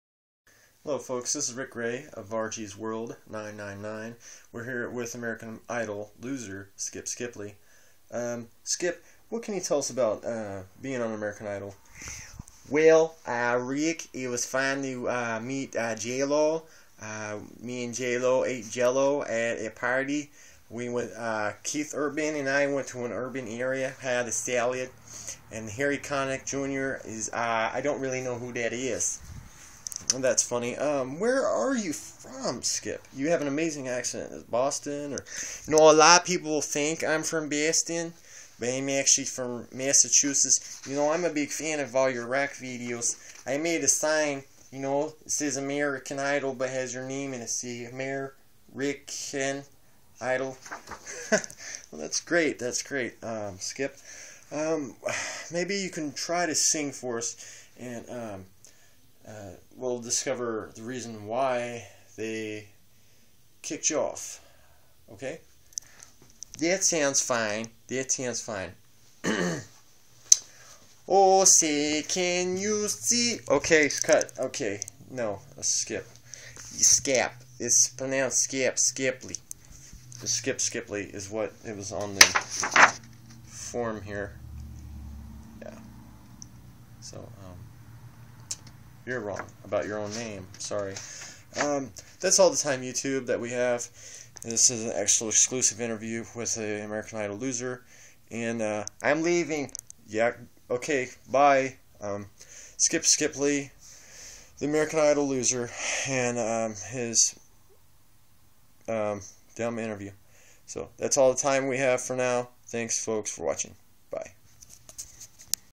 Interview with American Idol loser
Rick Gray interviews Skip Skipley who was kicked off American Idol. Exclusive interview!